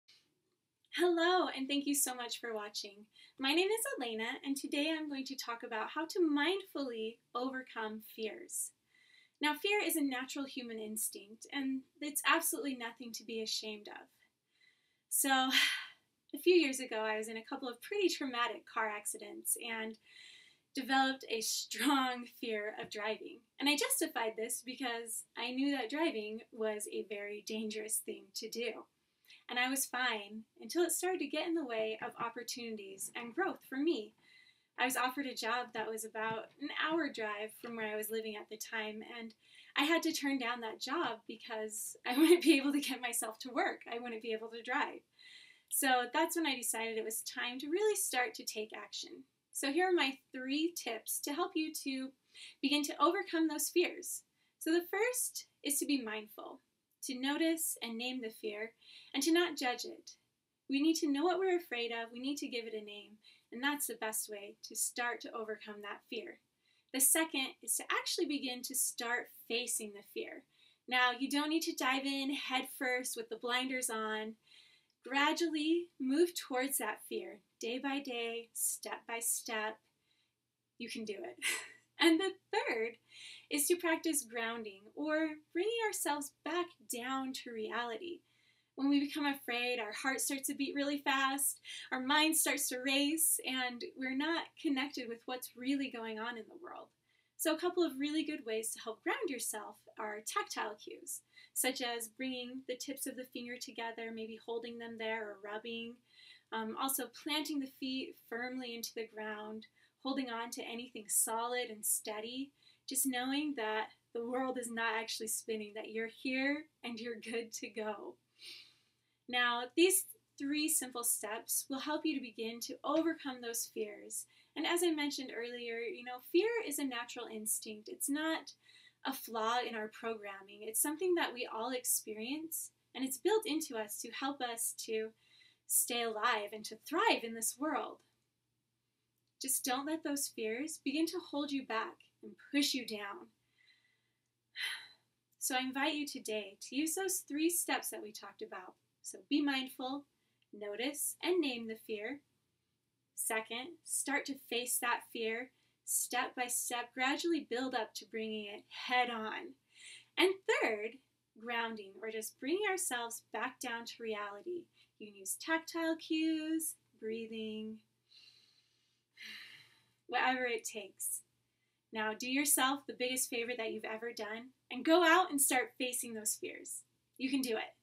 How to Mindfully Overcome Your Fears
My top three ways to beging to mindfully overcome your fears